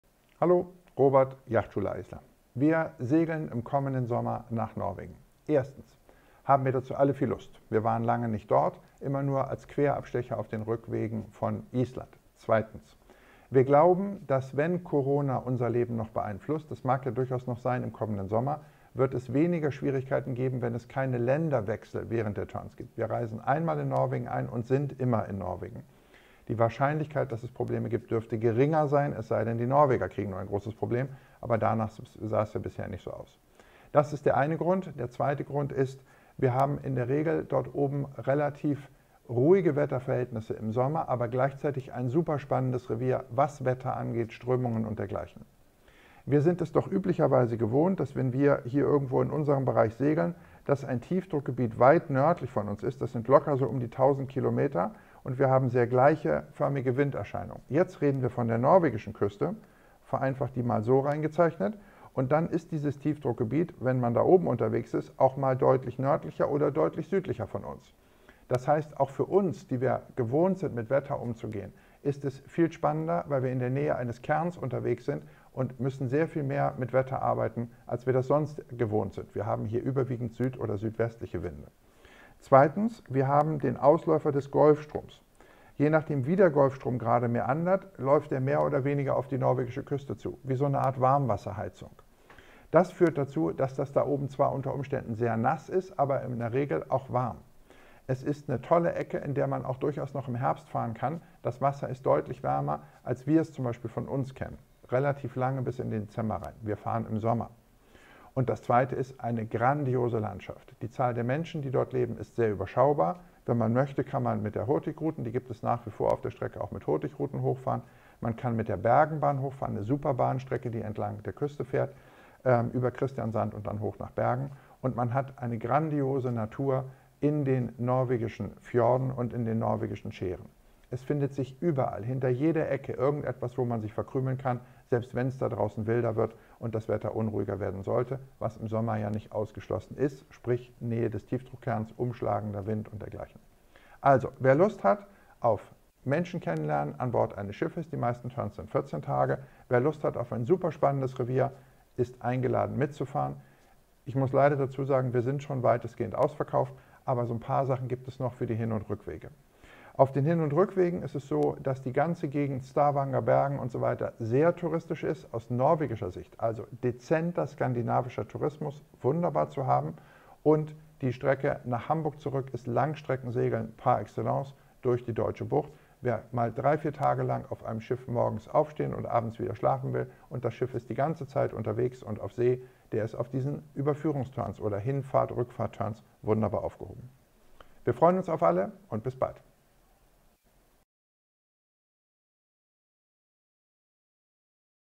Segeln zum Nordkap
Im nächten Sommer bieten wir Segeltörns in Norwegen an. Ein selbst für uns sehr spannendes Segelrevier. Was euch erwartet und was dieses Revier so besonders macht, erzählt euch Robert in diesem Video.